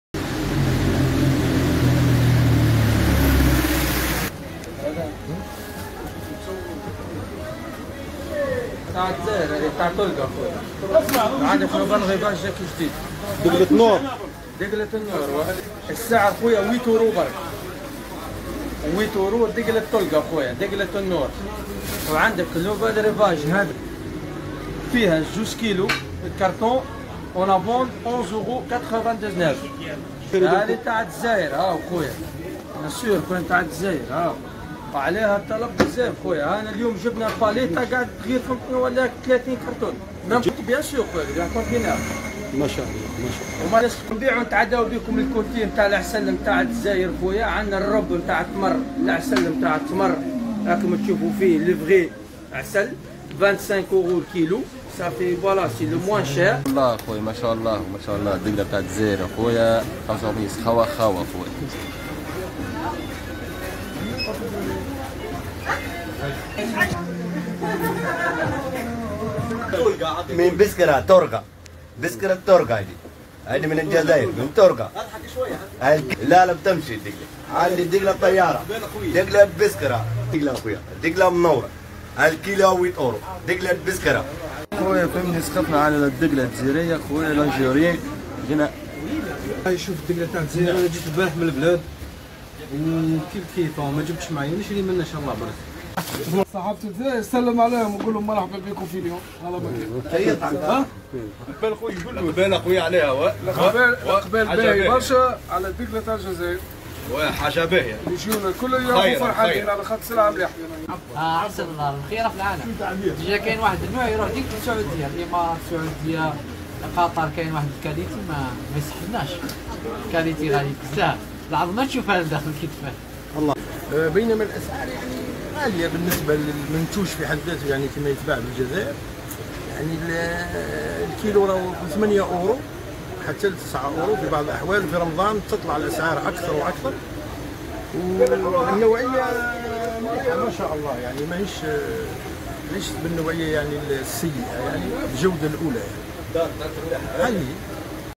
منع تمور جزائرية من اسواق الفرنسية و استبدالها بتمور ....تمور جزائرية
تمور جزائرية